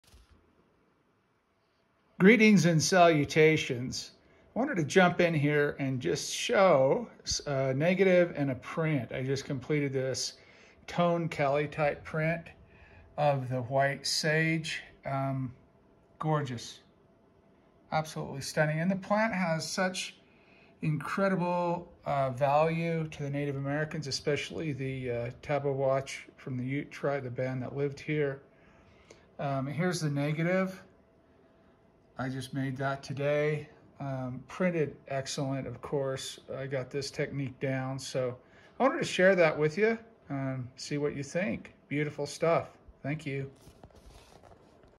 White Sage: Wet Collodion Negatives and Toned Kallitype Prints
"In the Shadow of Sun Mountain" - a project dealing with the land of the Ute/Tabeguache tribe in the Rocky Mountains of Colorado. White Sage: Wet Collodion Negatives and Toned Kallitype PrintsWet Collodion Negatives and Toned Kallitype Prints.